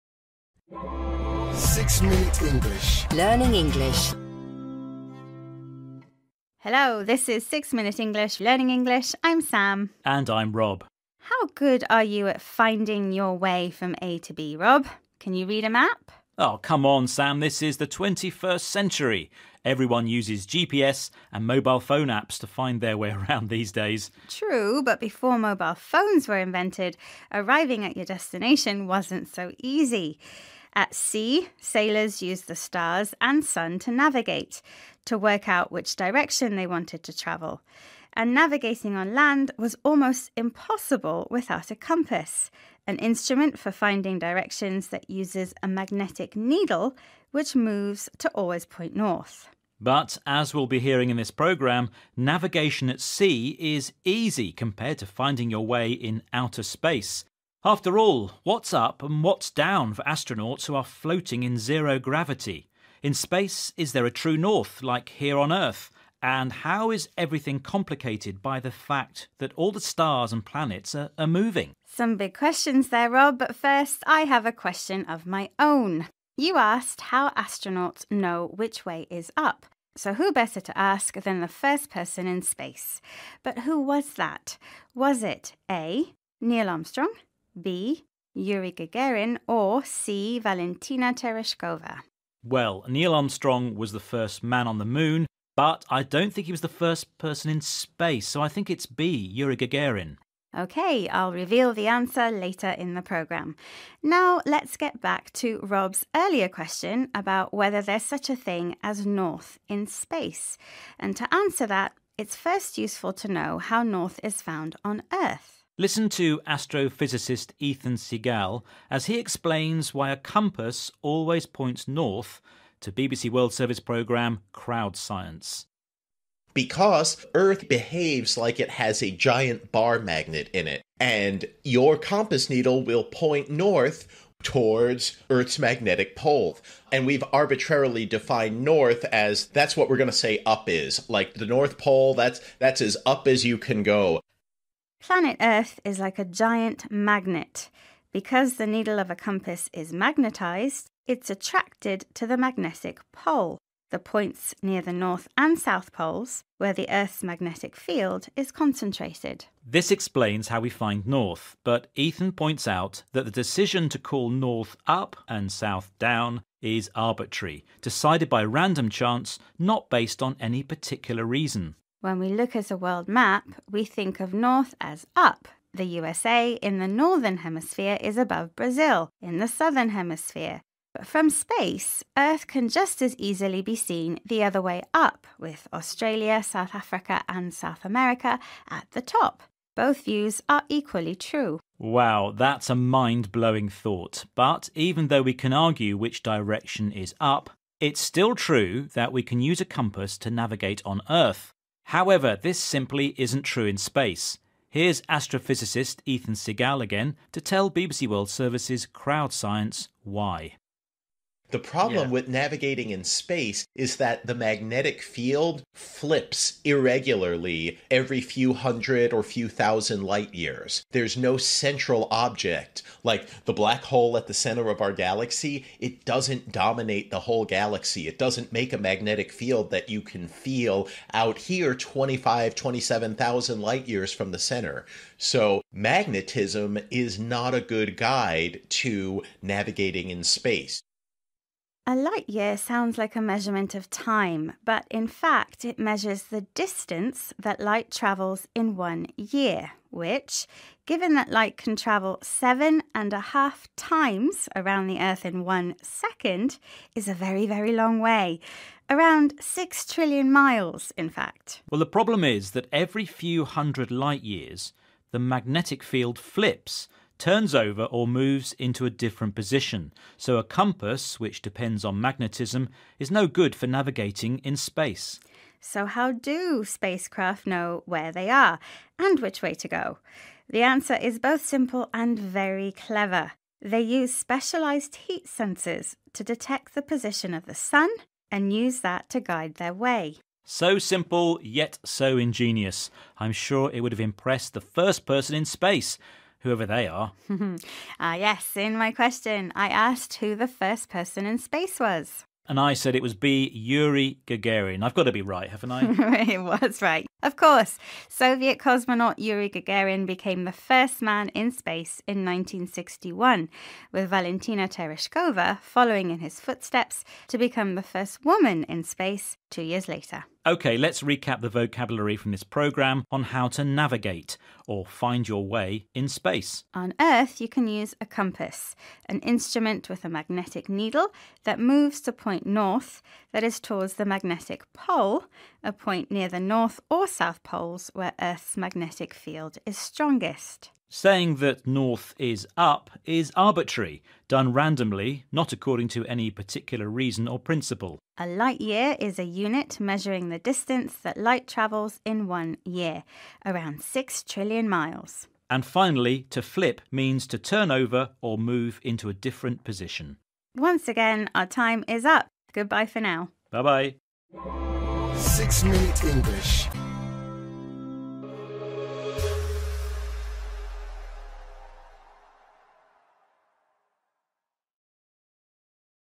Finding your way in space - Learning English everyday with Thomasdang
What’s up and what’s down for astronauts who are floating in zero gravity? In space is there a true north, like here on Earth? And how is everything complicated by the fact that all the stars and planets are moving? Sam and Rob talk about it and teach you related vocabulary along the way.

Who was the first person in space?
a)    Neil Armstrong
b)    Yuri Gagarin
c)    Valentina Tereshkova
Listen to the programme to find out the answer.

Vocabulary
navigate
use directions to go where you want to, often by using a map

compass
instrument for finding directions that works using a magnetic needle that moves and always points north

magnetic pole
point near the North Pole or South Pole where the Earth’s magnetic field is concentrated

arbitrary
decided randomly, not according to any particular reason or principle

light year
unit measuring the distance that light travels in one year (around 6 trillion miles)

flip
turn over or move into a different position